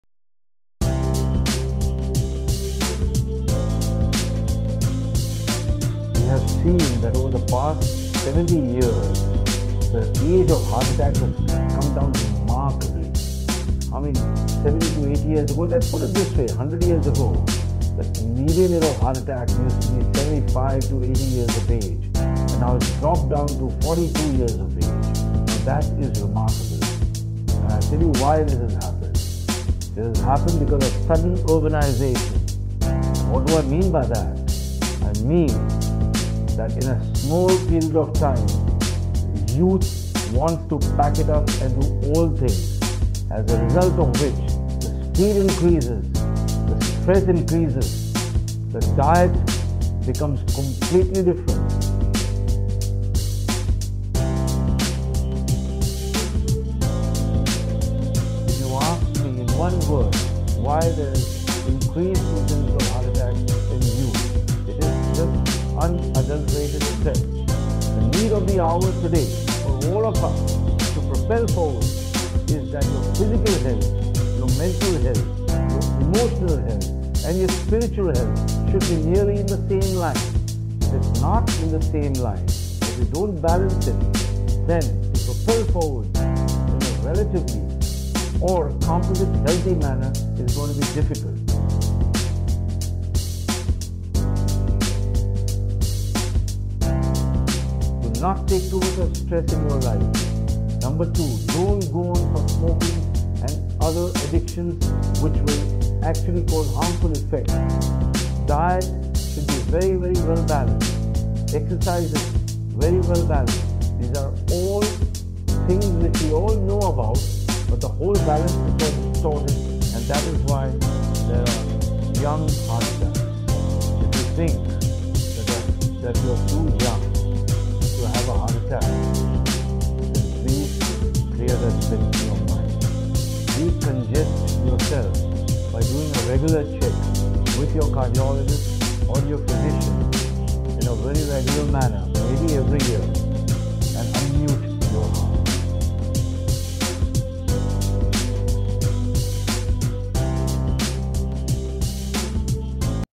Too Young for a Heart Attack? Think Again | Dr. Rajesh Rajani
Do you think you are too young to be at risk of a heart attack?​

The reality is, a modern lifestyle and genetic factors mean the silent epidemic is already here. Hear from our expert, Dr. Rajesh Rajani, Consultant Coronary Interventionist & TAVR Specialist , on why this video isn't for your parents it's for you.​

Watch to learn how to clear the congestion in your heart. UnmuteYourHeart​